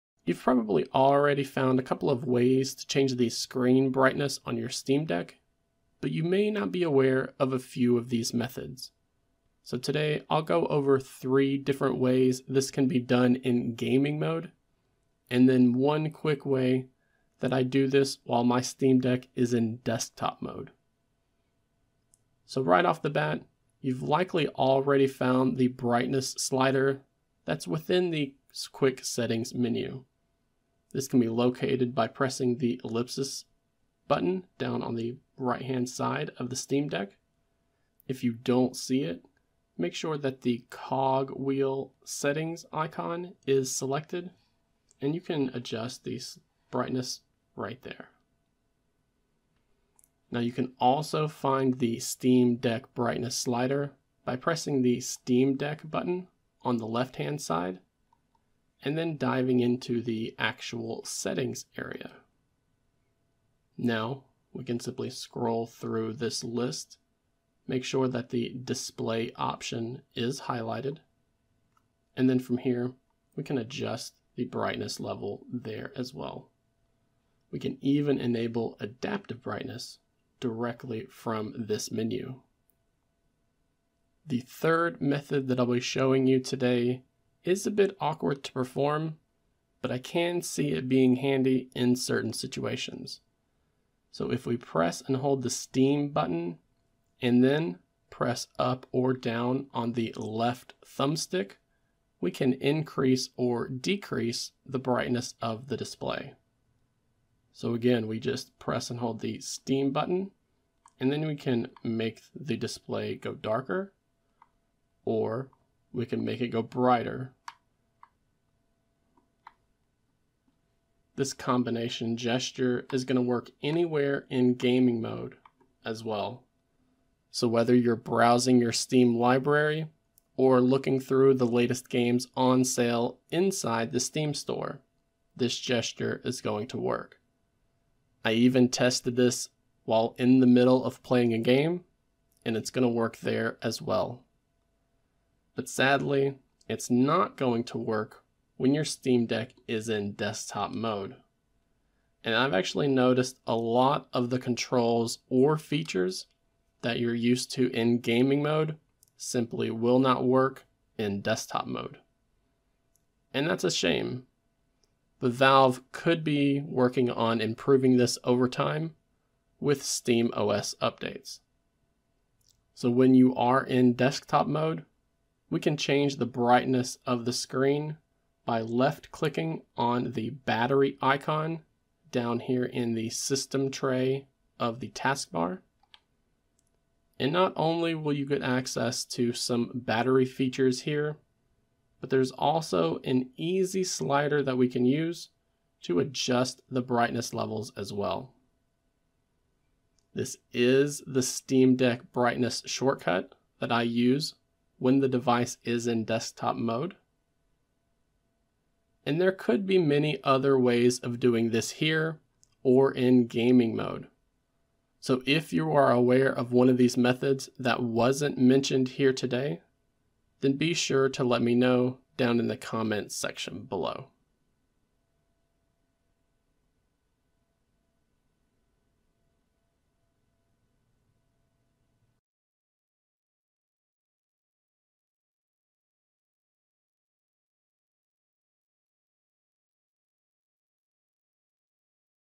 4 Ways to Change Steam Deck Brightness Levels | Including Desktop Mode
You're probably not aware there are 4 different ways to change the brightness of the Steam Deck, so let me go over them all.

Written Tutorial
coming soon!

Glass Screen Protector
TPU Case
Carrying Case
Portable Power Bank

Summary
You've probably already found a couple of ways to change the screen brightness on your Steam Deck, but you may not be aware of a few of these methods. Today, I'll go over three different ways this can be done in Gaming Mode and then one quick way to do this while it's in Desktop Mode.

So right off the bat, you've likely already found the brightness slider within the Quick Settings menu.
This can be located by pressing the Ellipse button here on the right side of the Steam Deck.
And then making sure the Cog Wheel Settings menu is highlighted.

This method is my go-to when in Gaming Mode on SteamOS or while I'm actively playing a game.
We can access anywhere except in Desktop Mode. So I'll get to that a bit later.

Now, you can also find the Steam Deck brightness slider by pressing the Steam button on the left side of the deck.
Then diving into the full Settings area.
Now just scroll through this list until you have the Display menu highlighted.
And again, you should easily be able to make the display brighter or darker.

The third method I'll be showing you today is a bit awkward to perform, but I can see it being handy in certain situations.
If we press and hold the Steam button, we can then press up or down on the left thumbstick to increase or decrease the brightness of the display.

This will work anywhere in Gaming Mode as well. So whether you're browsing your Steam Library or looking through the latest games on sale in the Steam store. This gesture will work.
I even tested this while in the middle of playing a game, and works there as well.

But sadly, it does not work when your Steam Deck is in Desktop Mode.
I've noticed that a lot of the controls or features in Gaming Mode simply do not work in Desktop Mode.
Which is a shame, but maybe Valve can improve this over time with SteamOS.

So when you're in Desktop Mode, we can change the brightness of the screen by left-clicking on the Battery icon down here in the system tray of the taskbar.

Not only will you get access to some battery features here, but there's also an easy to use slider to adjust the brightness levels as well. This is the Steam Deck brightness shortcut I use when the device is in Desktop Mode.

There could be many other ways of doing this here, or even in Gaming Mode.
So if you're aware of one that wasn't mentioned here today, be sure to let me know down in the comments section below.